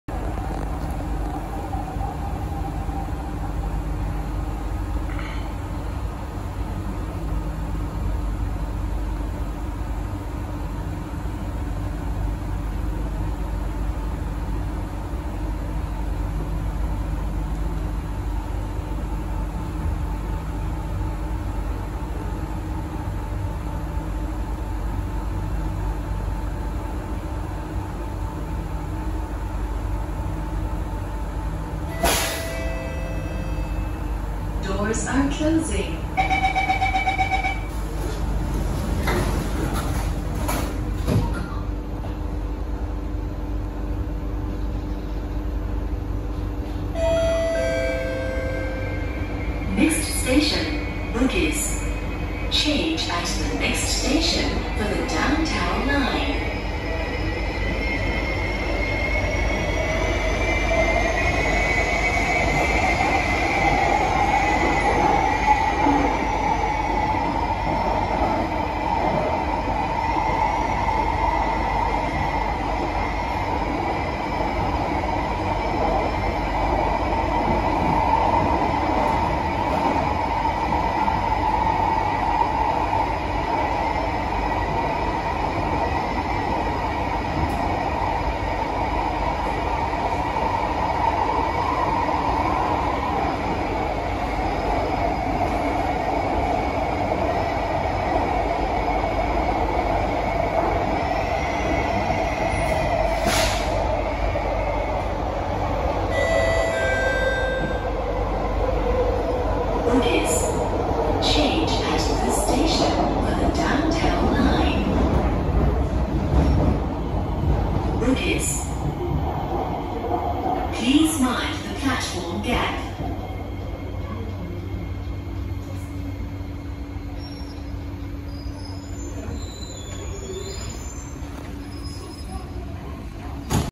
SMRT Trains EWL Kawasaki & CSR Sifang C151B [649/650] - Lavender to Bugis (➡️ Joo Koon)
Recorded on 24 April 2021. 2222hrs-2225hrs. I took over my previous C651 207/208 ride by taking C151B 649/650 instead. So I filmed from EW11 Lavender to EW12/DT14 Bugis for EW29 Joo Koon. At least the ride become so much better for me :)

Published on 8 June 2021, 2100hrs